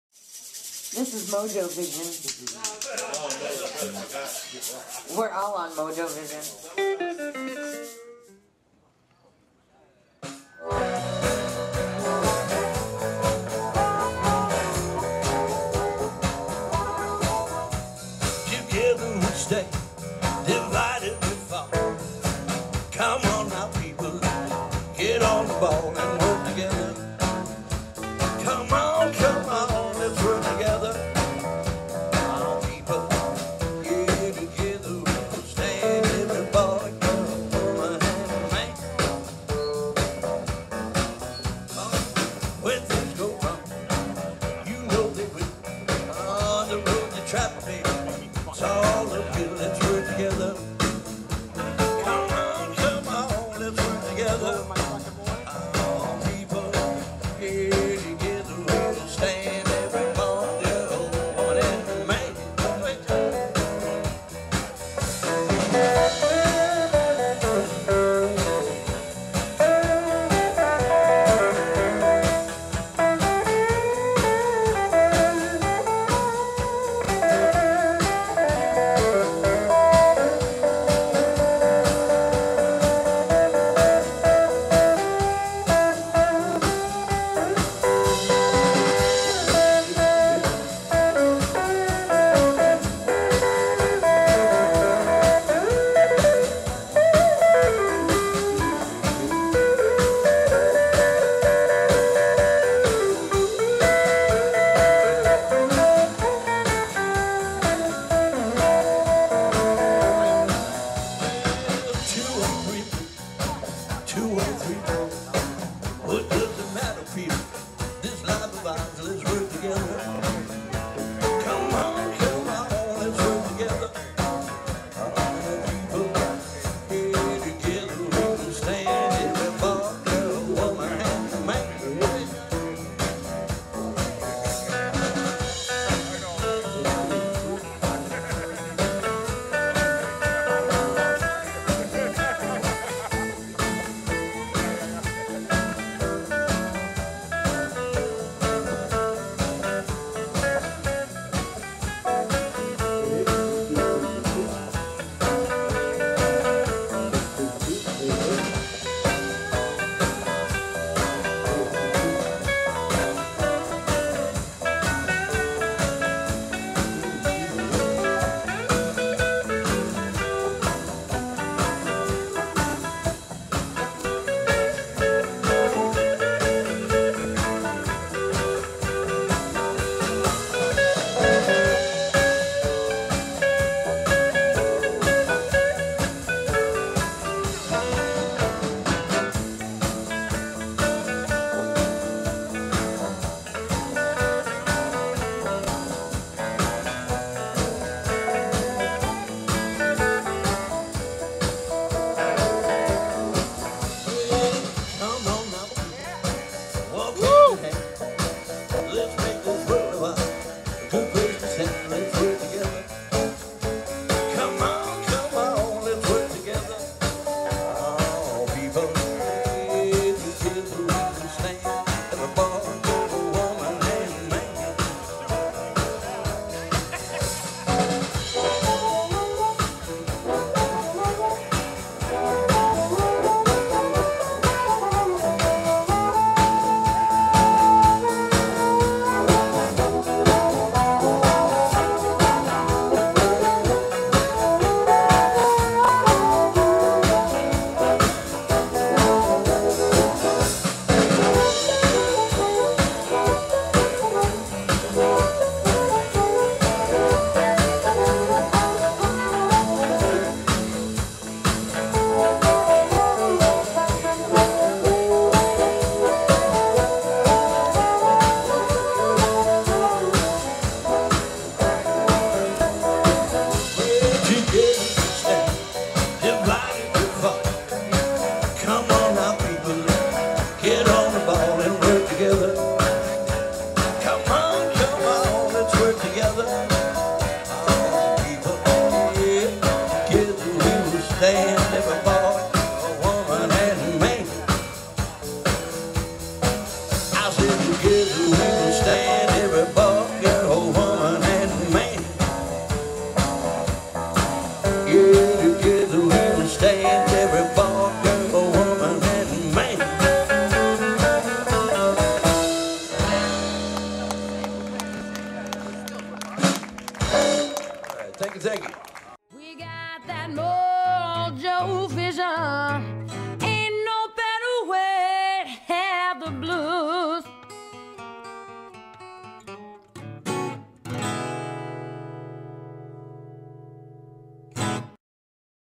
Johnny C & The Double Ds Live @ The 23rd Annual North River Blues Festival 8/26/18
John Capezzuto on Guitar
Tyler Caputo on Guitar
Douglas Banks on Drums
Matt McManamon  on Bass
Joe Curley on Harp